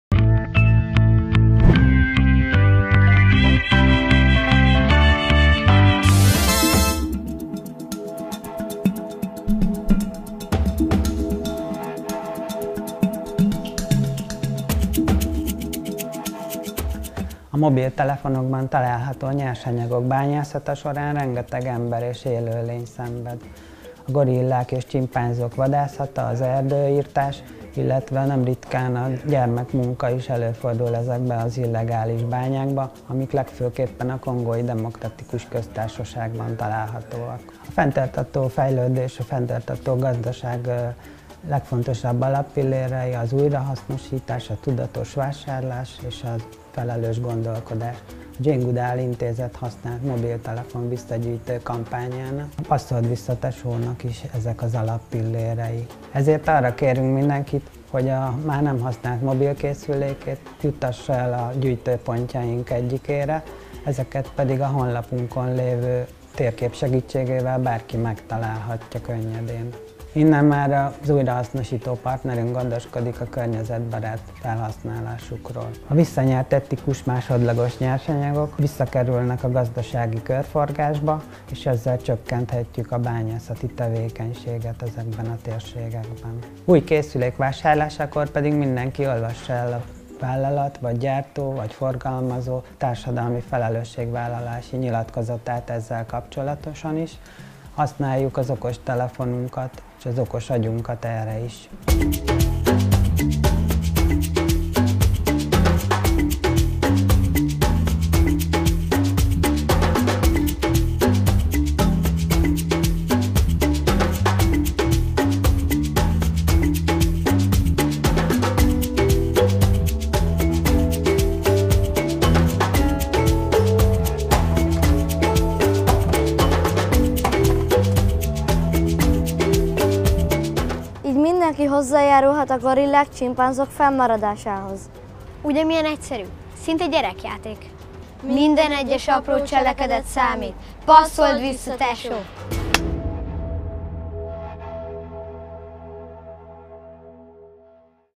DELFIN Díj 2019 fenntartható tudatformálás Jane Goodall Intézet
2019-ben a TELEKOM jóvoltából Delfin-díjat nyertünk a "Passzold vissza Tesó!" kampányunkkal. Köszönjük!